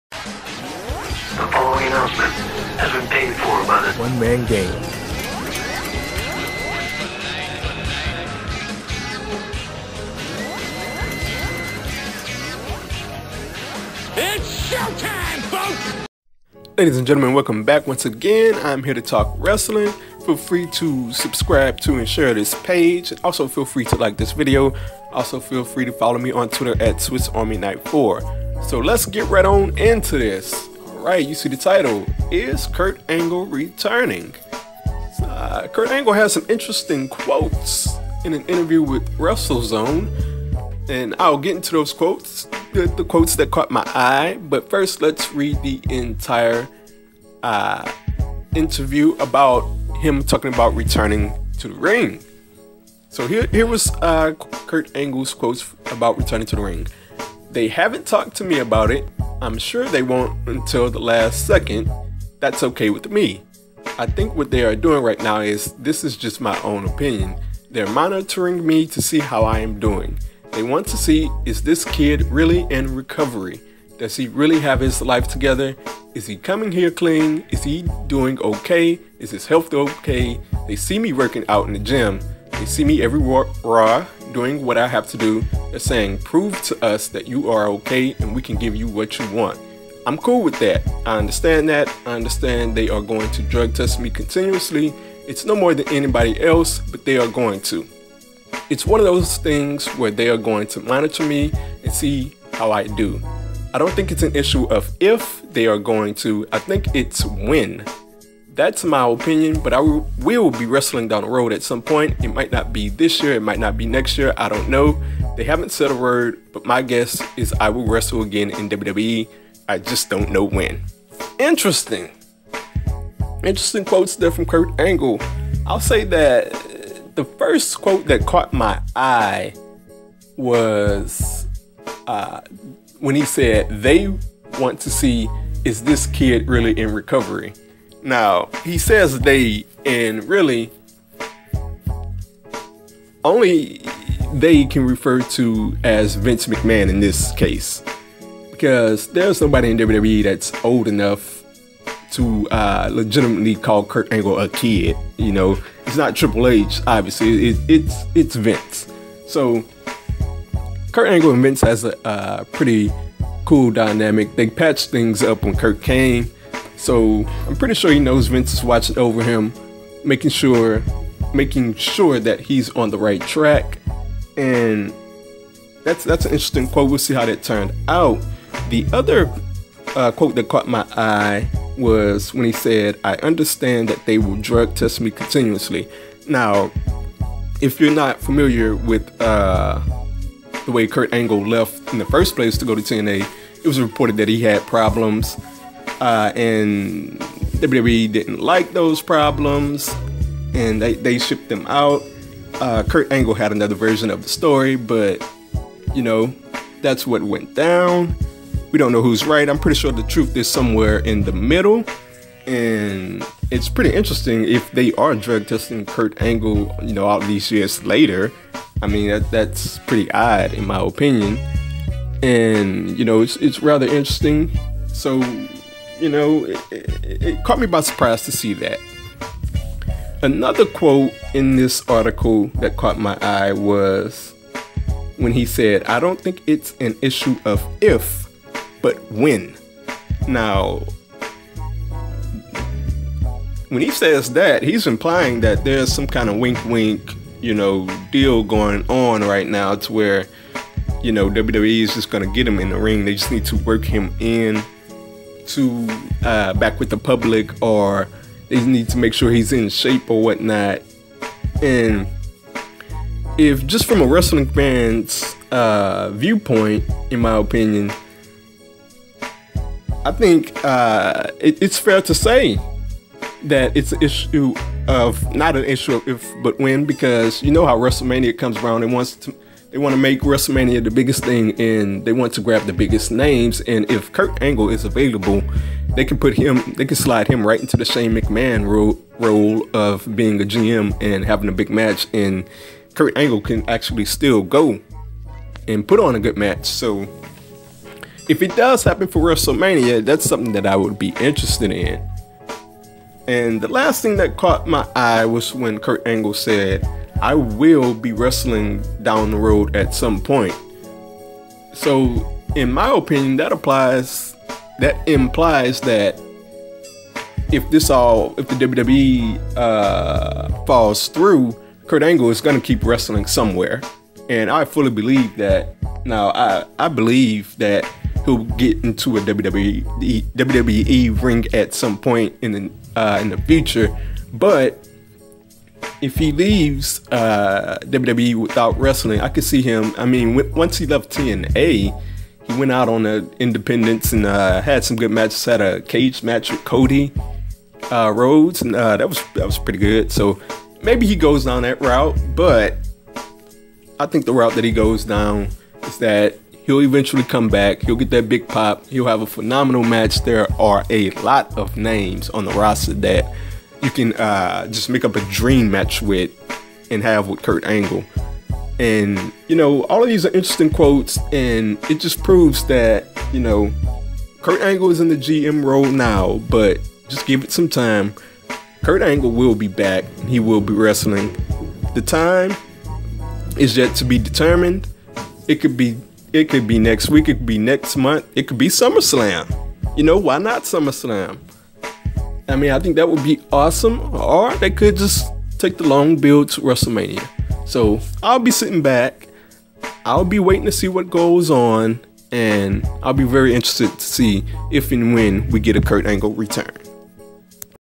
Is Kurt Angle returning to the ring?
Kurt Angle spoke with WrestleZone about a possible in ring return with WWE. My thoughts...